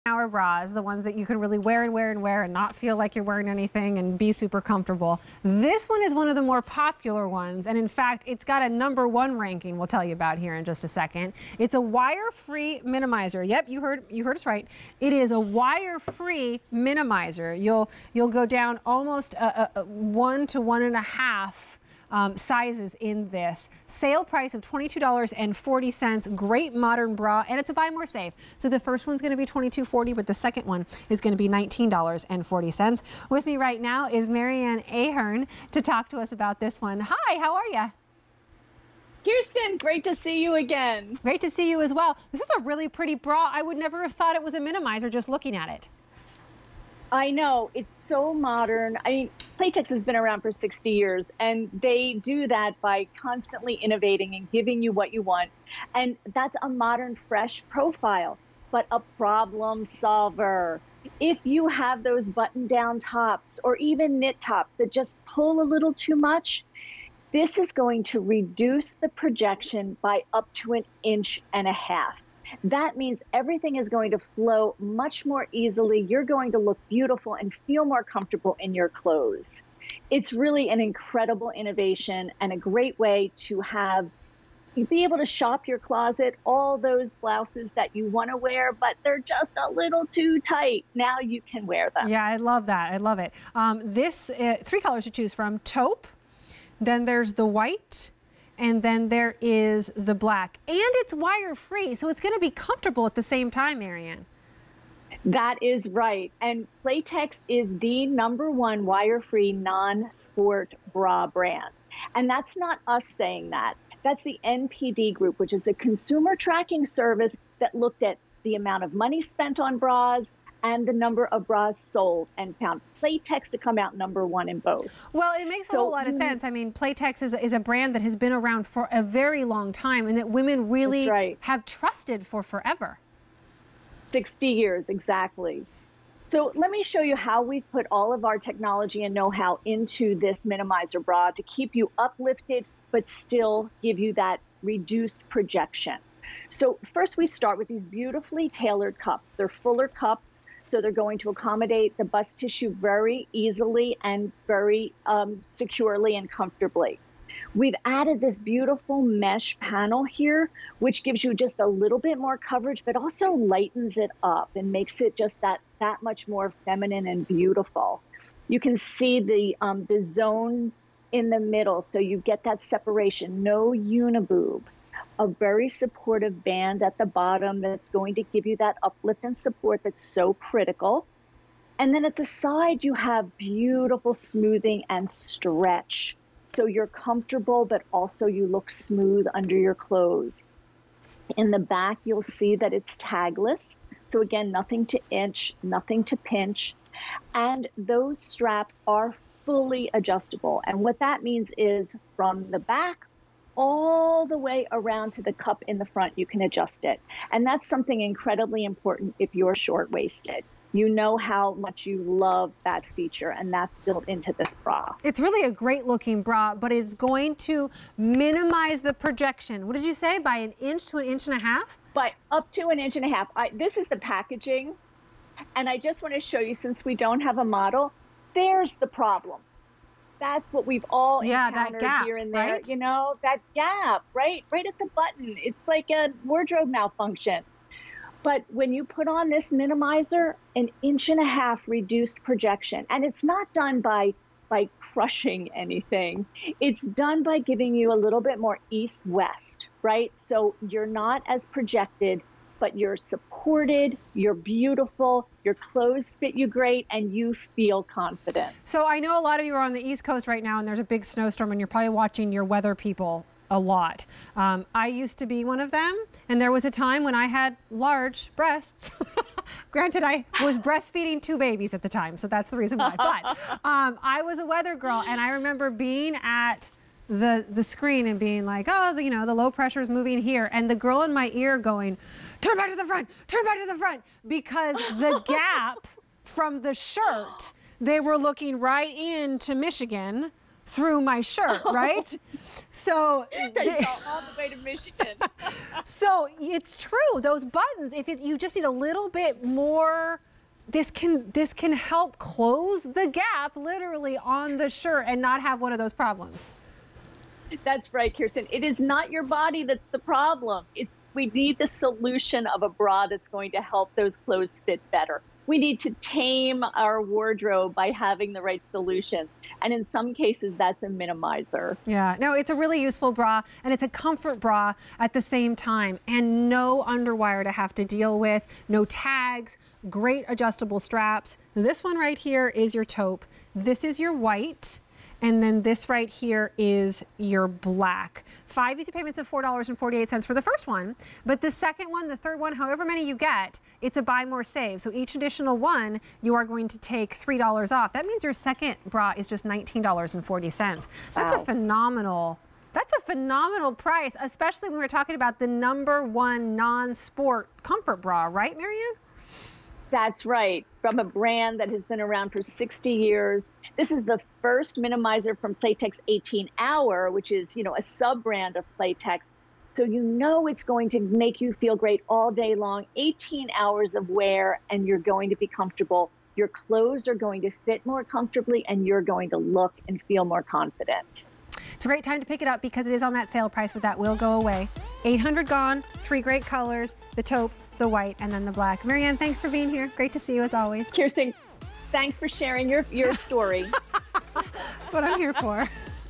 Playtex Smoothing Wirefree Minimizer Bra on QVC
Playtex Smoothing Wirefree Minimizer Bra

A wirefree bra that tames the bust area with frump-free full-coverage? This is the bra-bestie of our dreams. From Playtex.